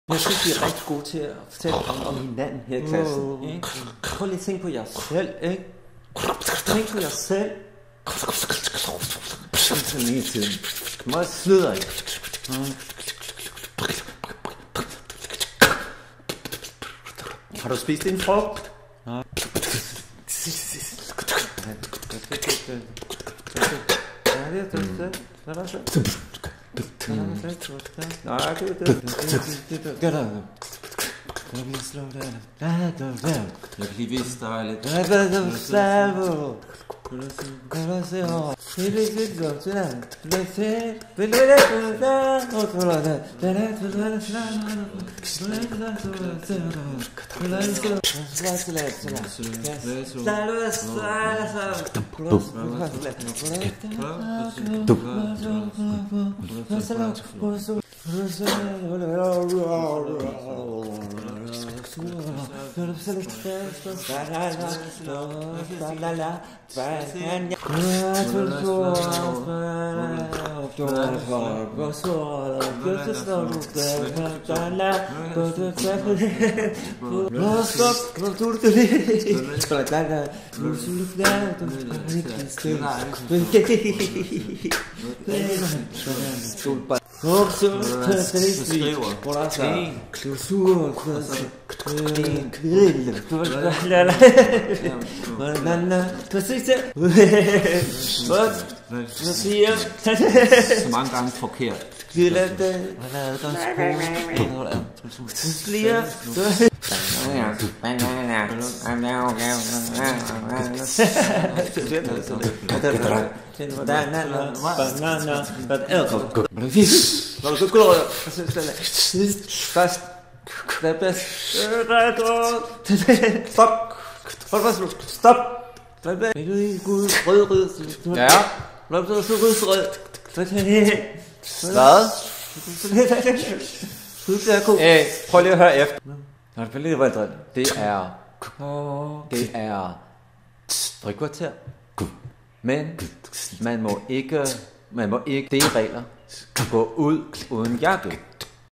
Corporeal analysis of field recording I
Can I use my body as an analytical tool? In this video, three  'beings' analyse different parts of the soundscape of a field recording. The recording was done in a school class with kids aged 7 - 8, in a morning break. One being is 'analysing' the noise sounds; a second one the high pitched voices; and a third one, the mid to low pitched voices. The original sound has been replaced by a vocal re-enactment, due to anonymity reasons, and as an extra layer of analysis.